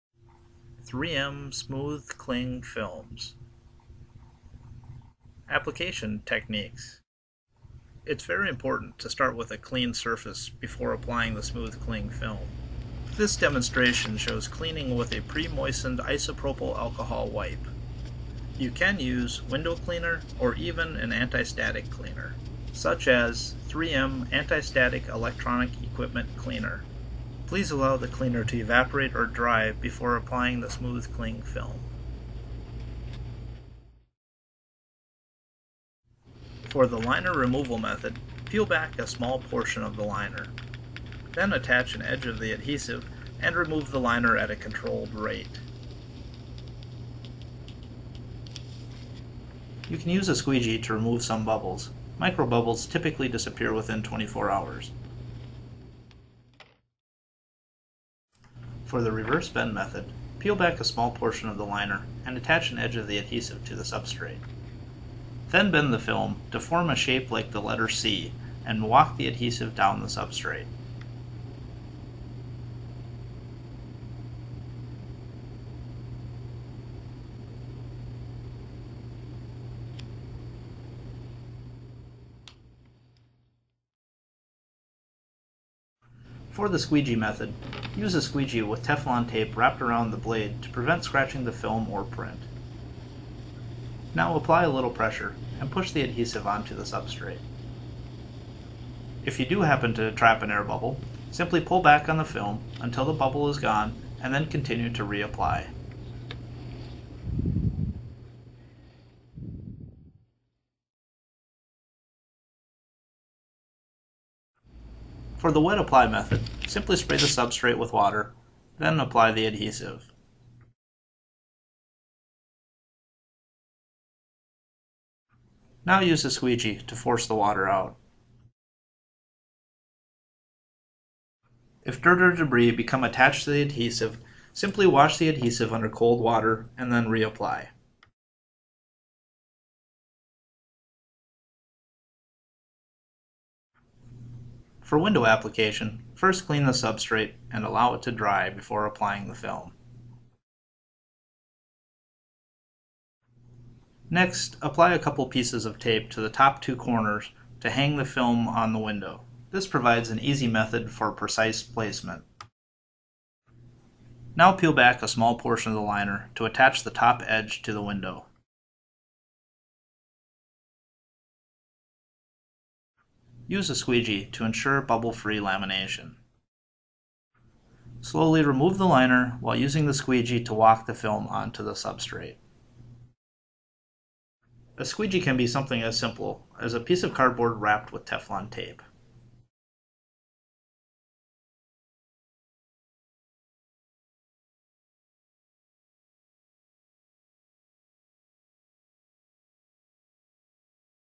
3M™ Smooth Cling Films Applications Techniques
Removable label material features unique self-wetting adhesive that flows fast with less bubbles than standard pressure sensitive adhesive. Labels go on smooth and secure without waiting or wiping.